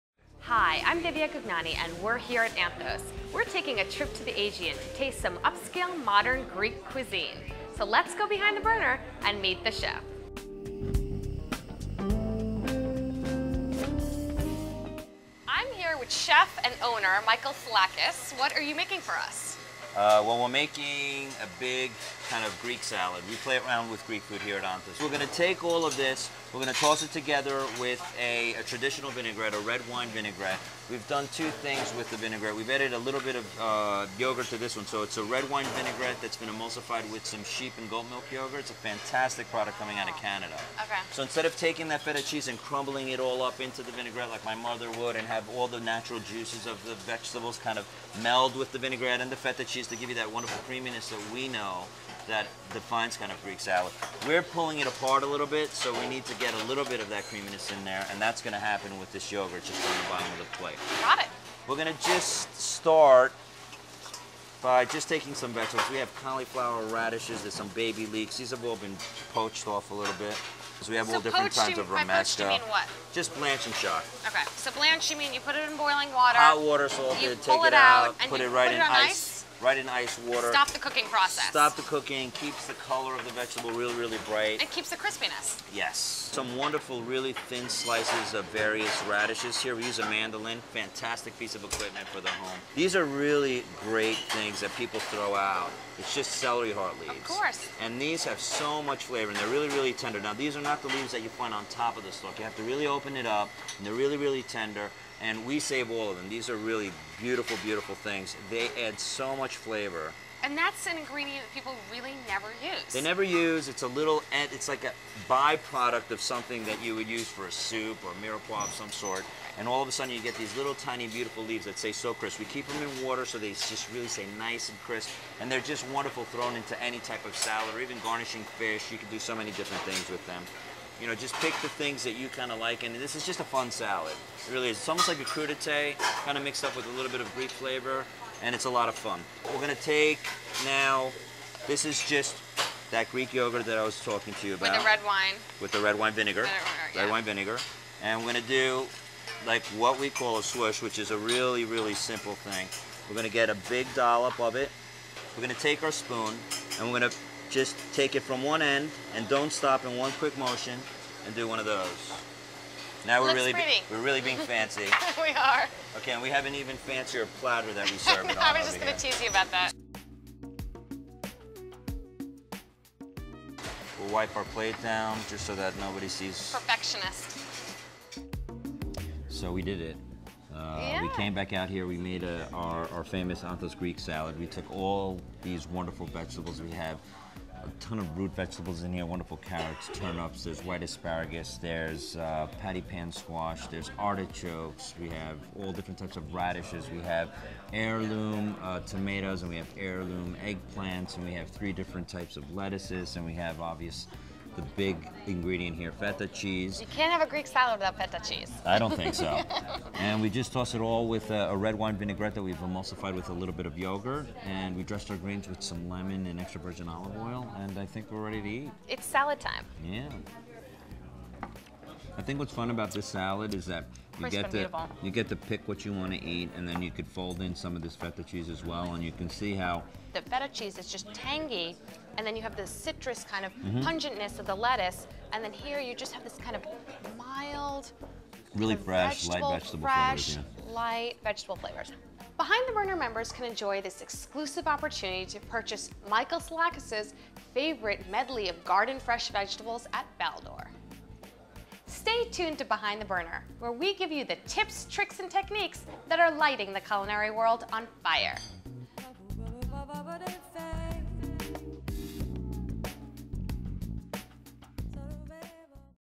Grecian Garden Vegetables w Chef Michael Psilakis at Anthos
Raw talent and tradition make for a nice seasoning from Chef Michael Psilakis. Greek yogurt emulsified with red wine vinaigrette, thinly sliced Baldor vegetables, and a glass of ouzo completes a meal for the gods.